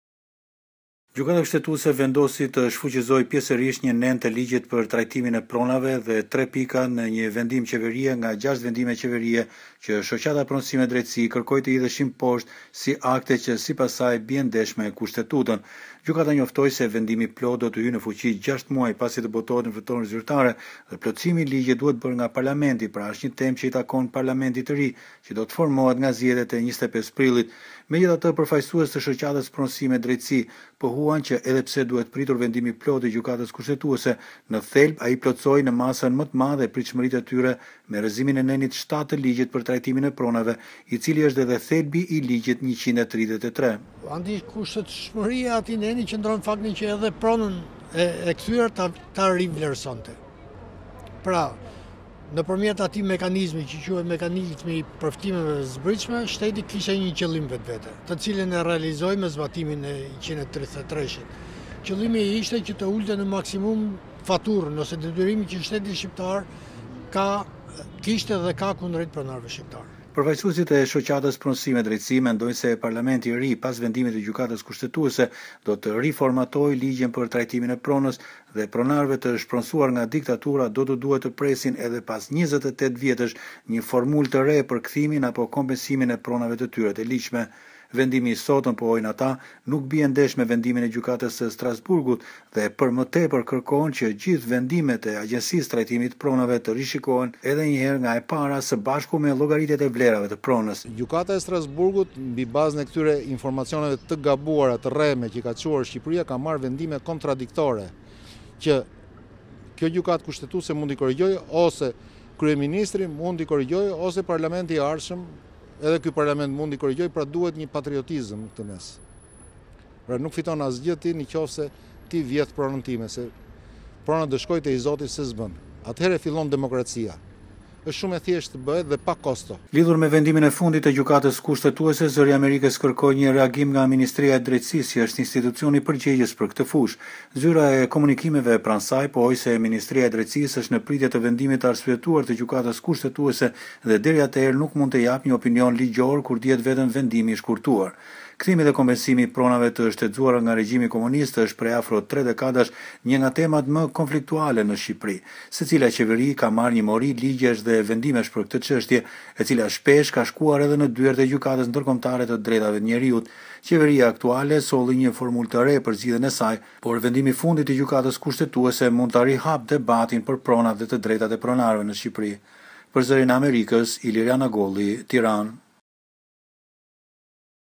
Gjykata Kushtetuese vendim mbi çështjen e pronave të shfrytëzuara nga diktatura
Në Tiranë, Gjykata Kushtetuese shfuqizoi si të papajtueshme me Kushtetutën një nen të ligjit për trajtimin e pronës dhe disa pika në një vendim qeverie, që lidhen me mënyrat e kthimit dhe kompensimin e pronave. Ky vendim rihap edhe një herë diskutimet mbi çështjen e pronave, për të cilat kanjë mori konfliktesh në Shqipëri. Padinë e kishte ngritur disa vjet më parë shoqata Pronësi me Drejtësi, e cila kërkon njohjen dhe kthimin e pronave private të shtetëzuara nga regjimi komunist.